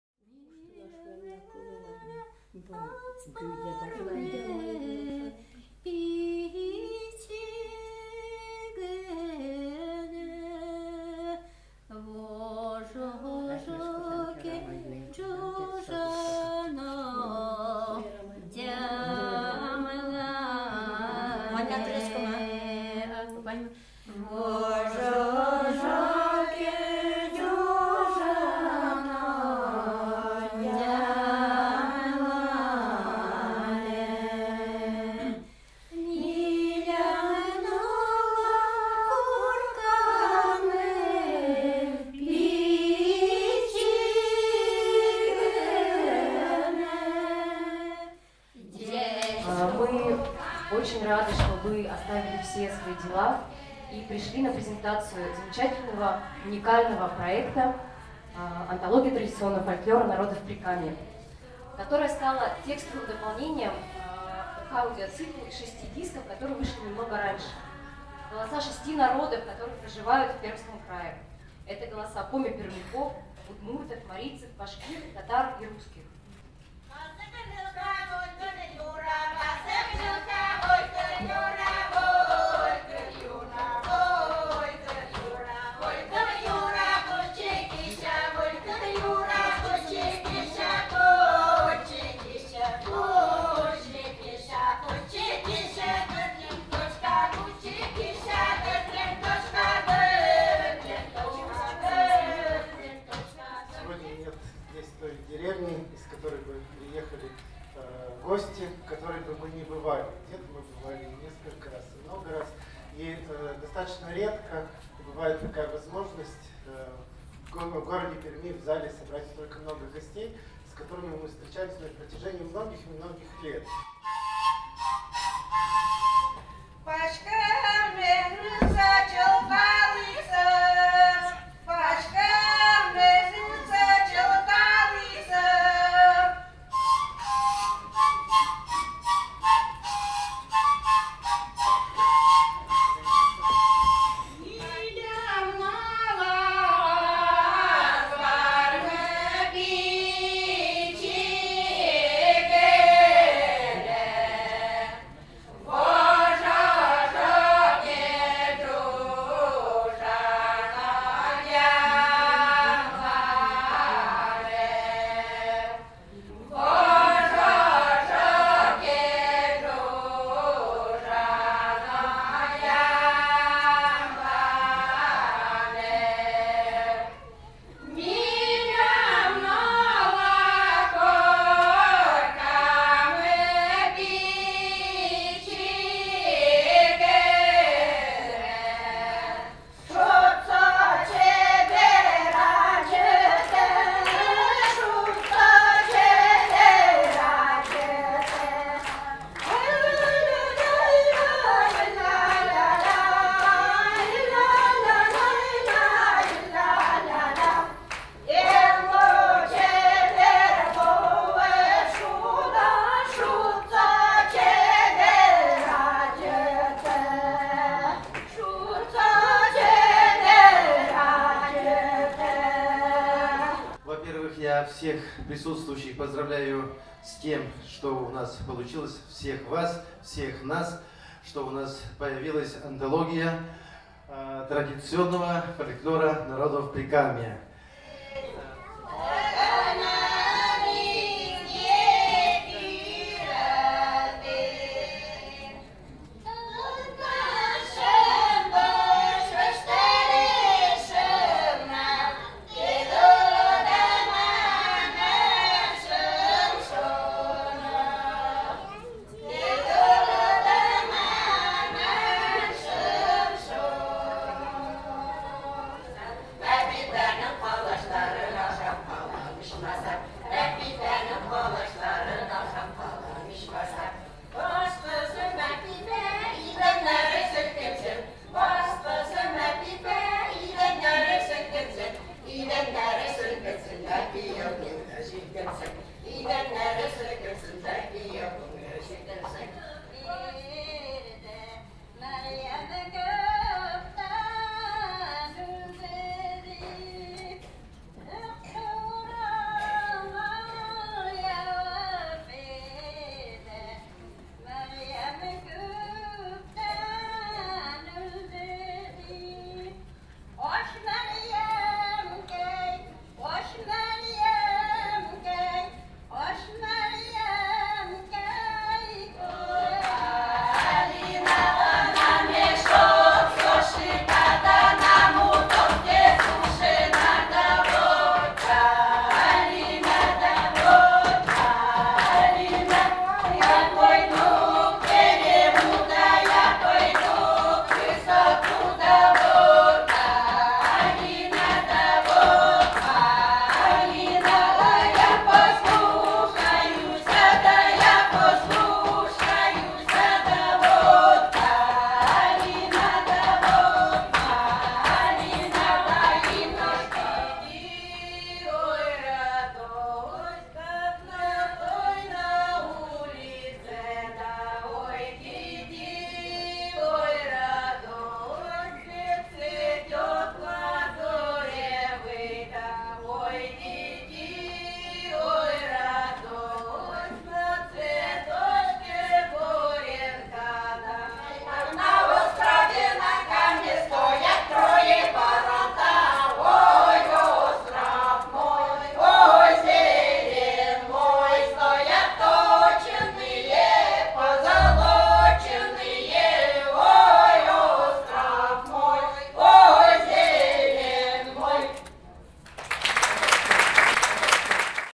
Презентация проекта «Антология традиционного фольклора народов Прикамья». Видео-архив 2014
Проект «Антология традиционного фольклора народов Прикамья» был инициирован ПРОО "КАМВА" с целью сохранения и включения в современный общественный контекст наследия коренных народов Пермского края. Удмурты, башкиры, татары, марийцы, русские, коми-пермяки издавна проживают на территории Пермского края, и переплетения этих культур создают неповторимое полотно  этнокультурной картины региона.
В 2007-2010 годах продюсерская группа KAMWA совместно с этнографами и фольклористами организовала научные экспедиции по селам и деревням Пермского края, провела аудиозапись исполнителей и аутентичных фольклорных ансамблей. Более 80 поселений в 18 районах и более 1000 исполнителей приняли участие в этом проекте. Результатом стал выпуск шести компакт-дисков, каждый из которых  представляет фольклор коренного народа Прикамья.
Следующим этапом проекта стала работа над  расшифровкой и переводами текстов народных песен на русский и английский языки. После 3 лет работы ученых, этнографов, филологов и лингвистов была издана книга «Антология традиционного фольклора народов Прикамья». Теперь песни, сказки и легенды, записанные во время экспедиций, собраны под одной обложкой. Книга уникальна тем, что впервые читатель может познакомиться с народной поэзией на родном языке исполнителя и в русском или английском переводе.

Презентация проекта состоится 26 февраля 2014 г. В концертно-выставочном зале отеля «УРАЛ» (Пермь, ул. Ленина, 58). На презентации присутствовали аутентичные коллективы из районов Пермского края, представители национальных сообществ и культурной администрации.
Создание медиатеки ПРОО "КАМВА" реализуется победителем конкурса"Общее дело" благотворительной программы "Эффективная филантропия" Благотворительного фонда Владимира Потанина